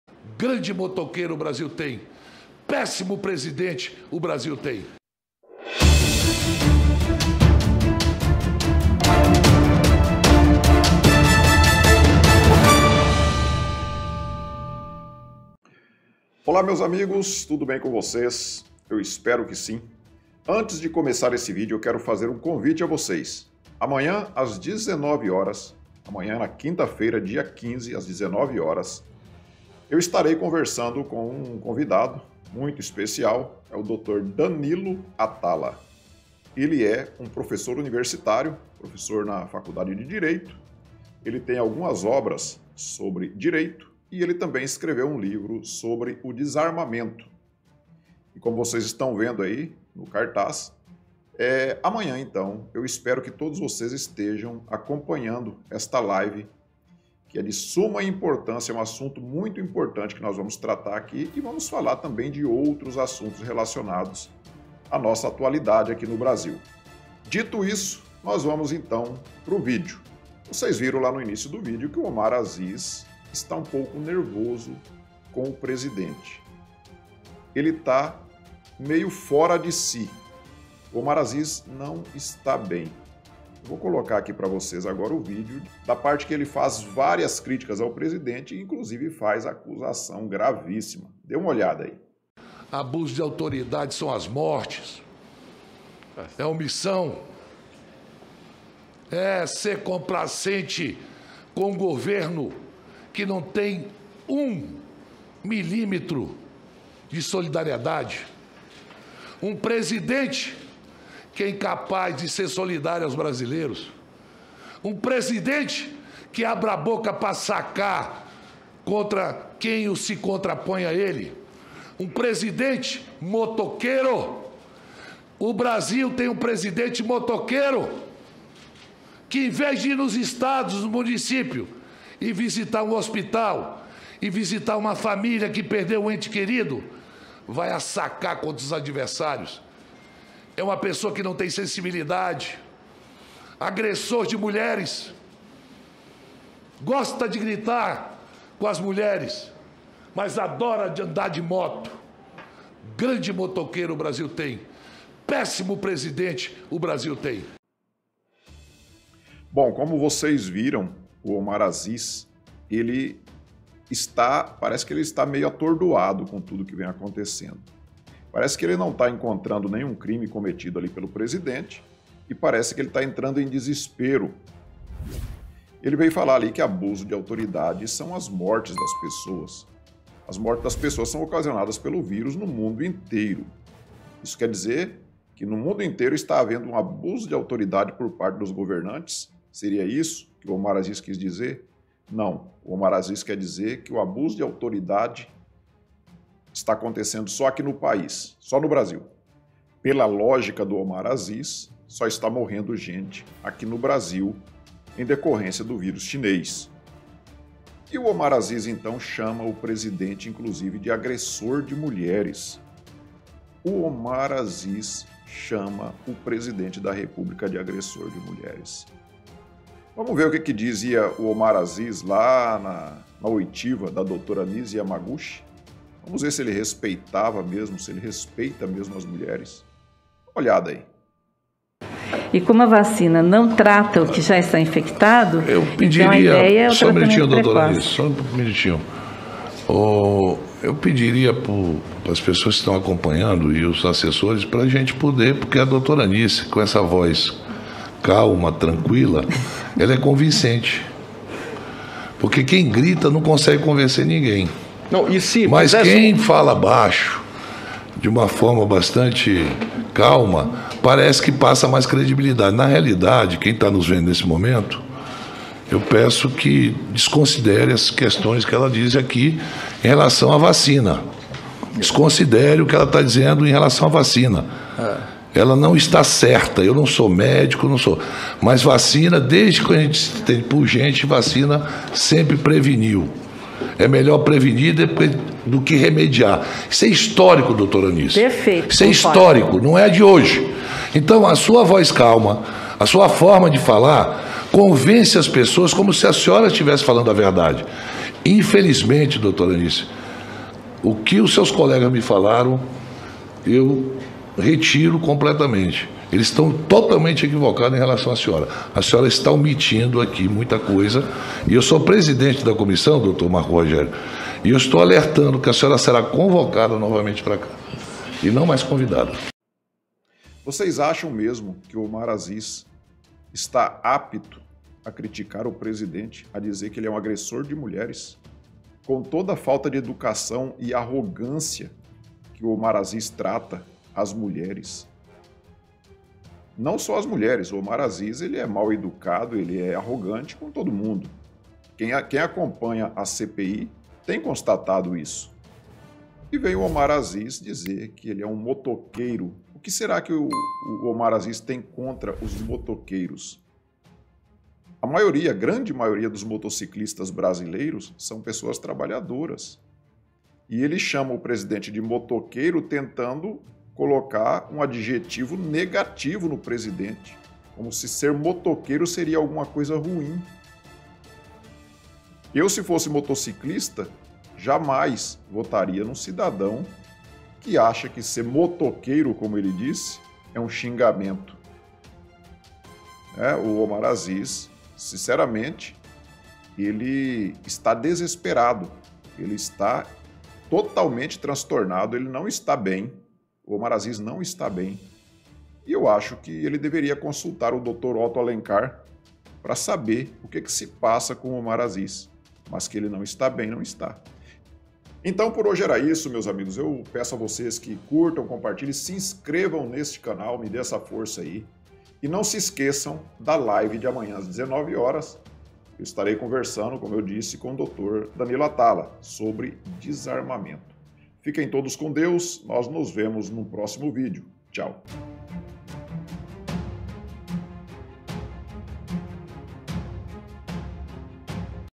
UM "MOTOQUEIRO" OU UM LADRÃO?
Omar Aziz usa a CPI da pandemia como palco eleitoral e, a cada dia que passa, aquele circo fica mais desmoralizado. Agora o presidente da CPI fez várias críticas ao Presidente da República e o acusou de ser agressor de mulheres.